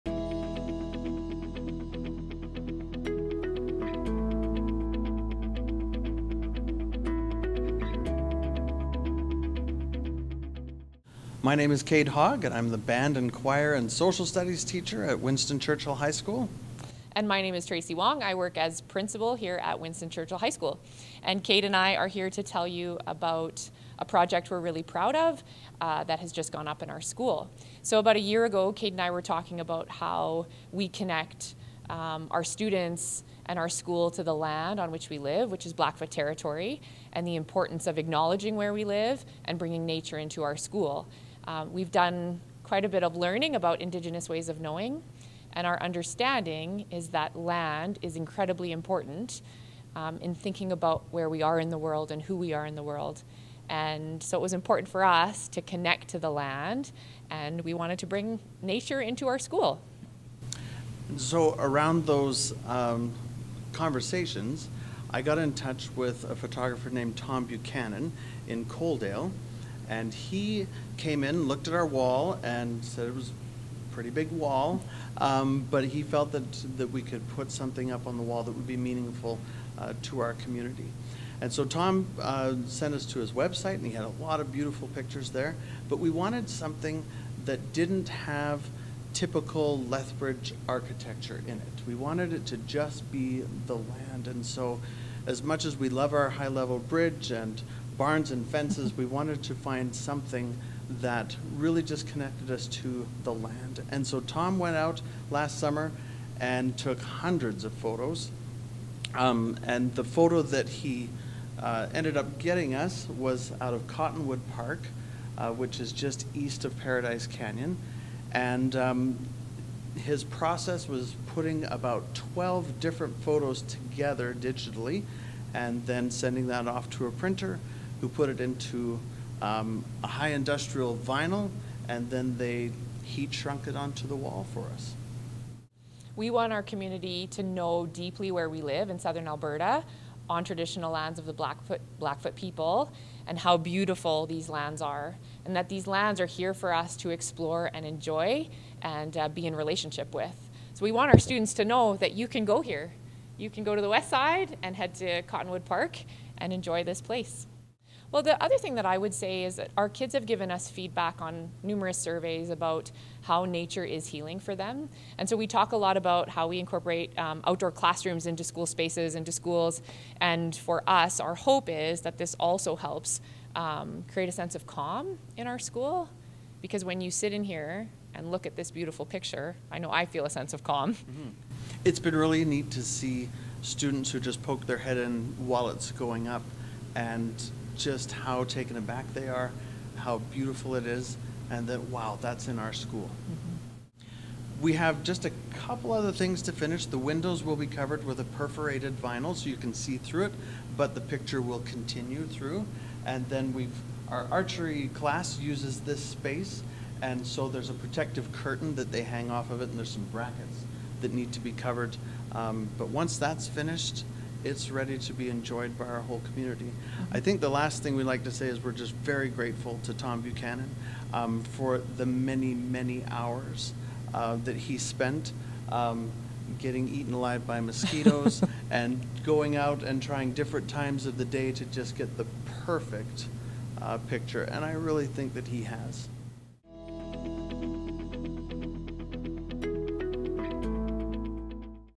WCHS Photo Installation
It is not everyday when the natural wonder of Lethbridge is emblazoned on a wall inside a school. But this month, Winston Churchill High School is completing a massive scale installation of a photograph that features Cottonwood Park.